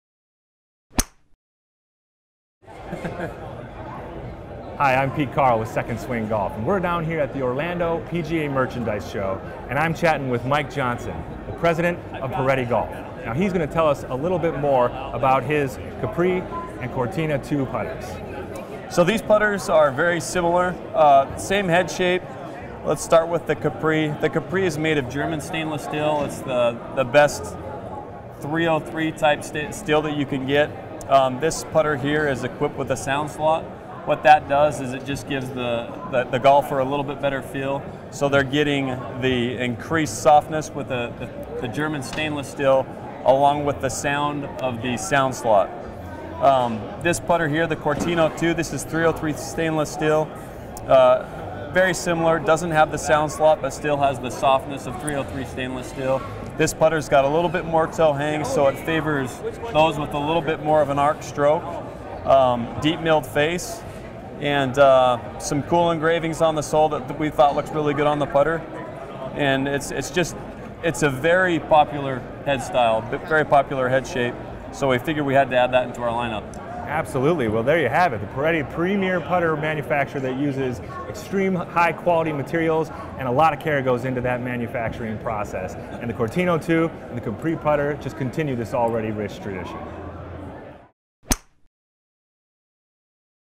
Piretti Capri and Cortino 2 Putters - 2nd Swing Golf
Piretti Capri Putter and Cortino 2 Putter

2nd Swing Golf club specialist Pete Karle talks to Mike Johnson, President of Piretti Golf, about the Piretti Capri and Cortino 2 putters.